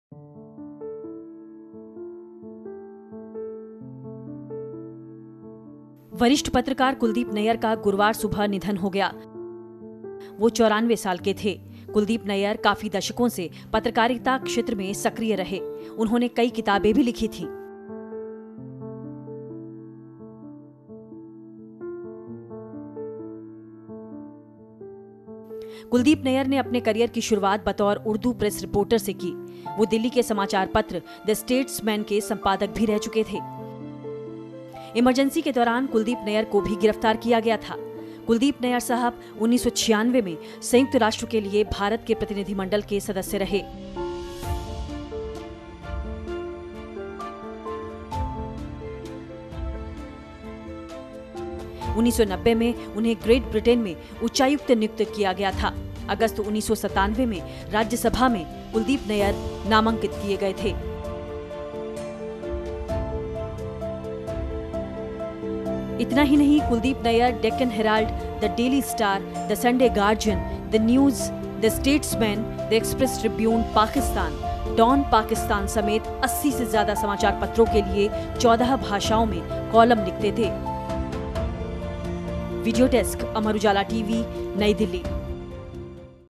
वरिष्ठ पत्रकार कुलदीप नैयर का निधन, आपातकाल में गए थे जेल
वरिष्ठ पत्रकार कुलदीप नैयर का गुरुवार सुबह निधन हो गया। वह 94 वर्ष के थे। कुलदीप नैयर काफी दशकों से पत्रकारिता श्रेत्र में सक्रिय थे। उन्होंने कई किताबें भी लिखी थीं।